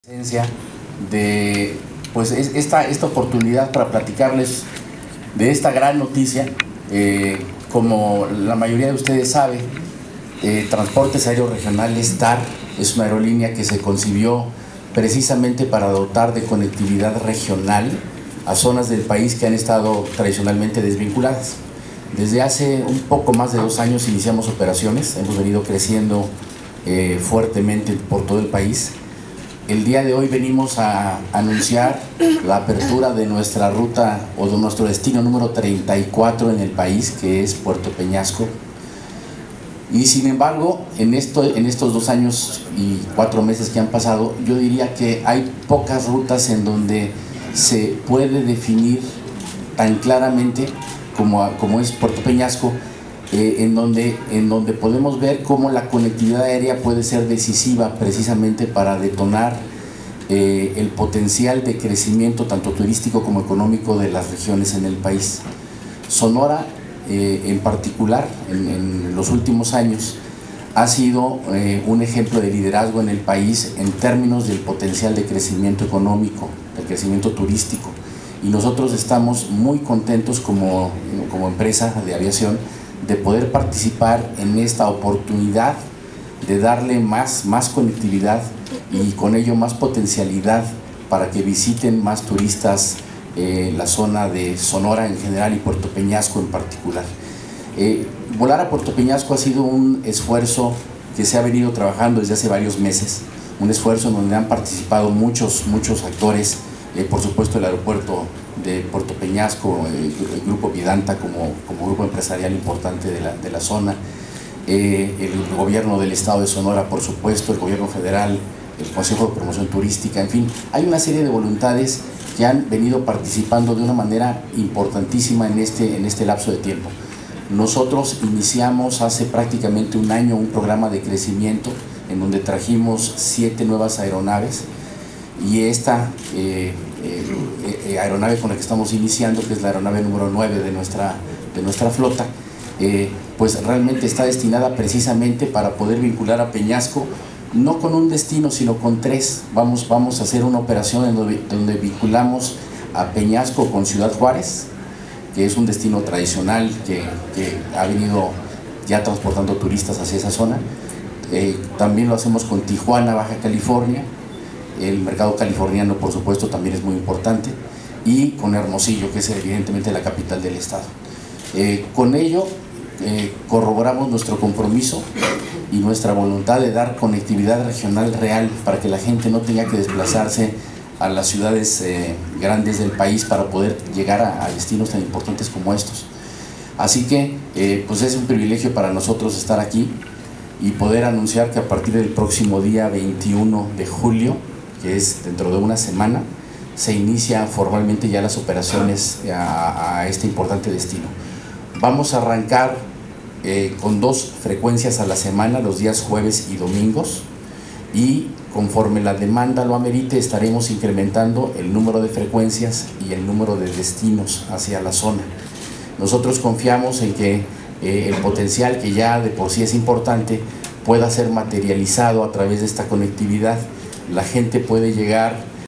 TAR tarifas bajas a Pto. Peñasco
Rodrigo Vásquez Colmenarez , ector general de TAR aerolíneas, explica  su confianza en el desarrollo de Sonora y la atracción de mayores inversiones